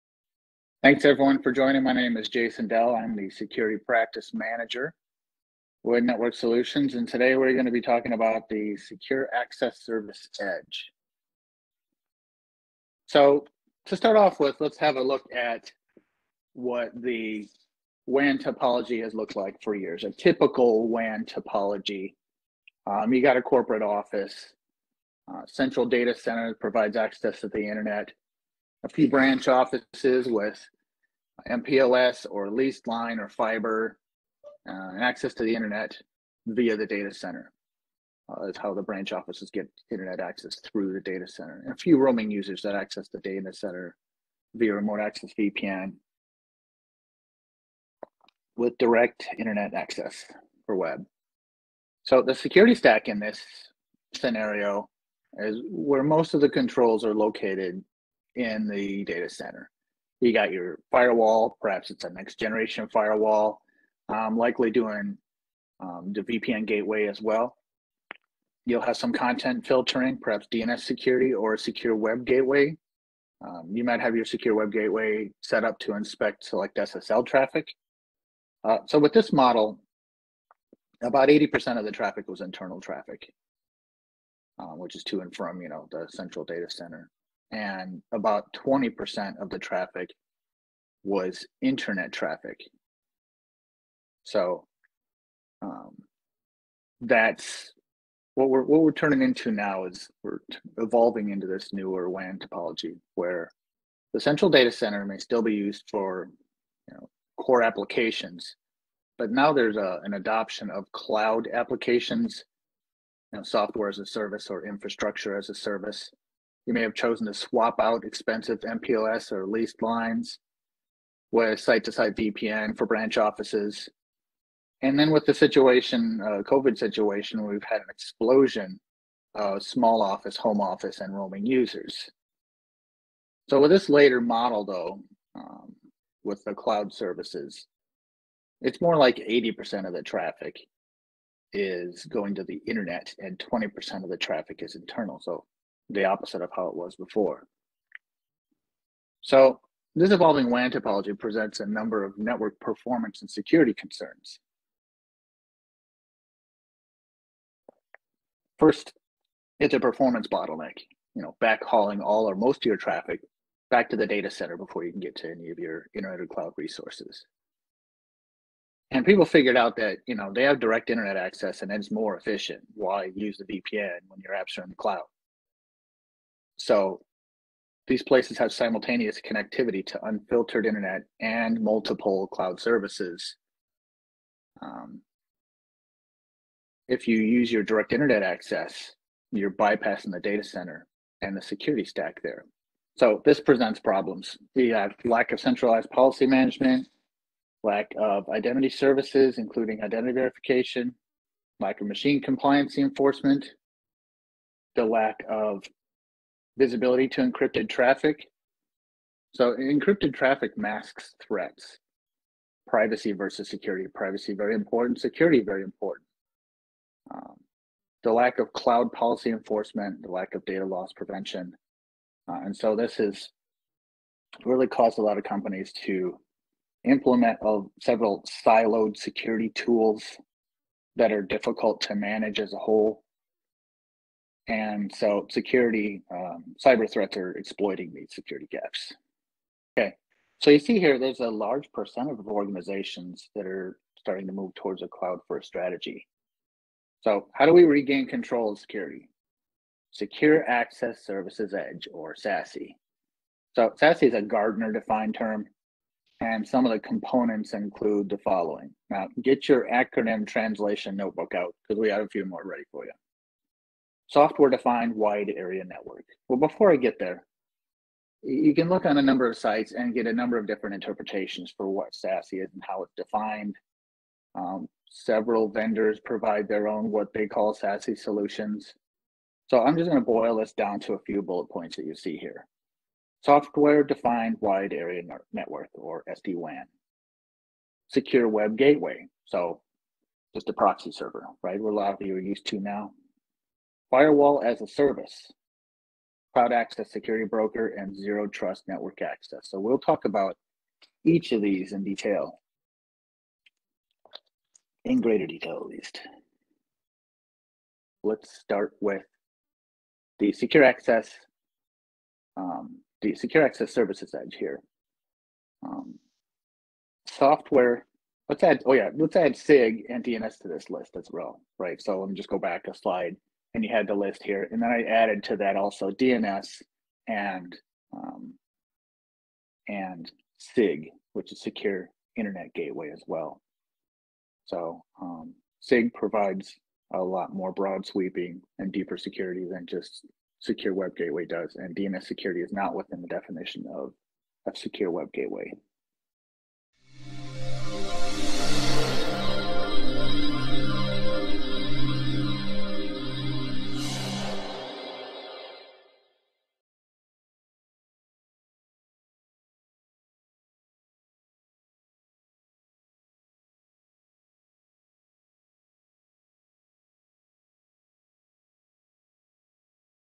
Solving WAN Vulnerabilities using SD-WAN, SWG, ZTNA
In Part 2 of this SASE (Secure Access Service Edge) presentation, Jason Dell explains how SD-WAN, Secure Web Gateway (SWG) and Zero Trust Network Access (ZTNA) mitigates some of the vulnerabilites associated with a large Work From Home WAN environment.

To learn more about Network Solutions' security capabilities or to download our ebook "Developing an Information Security Strategy"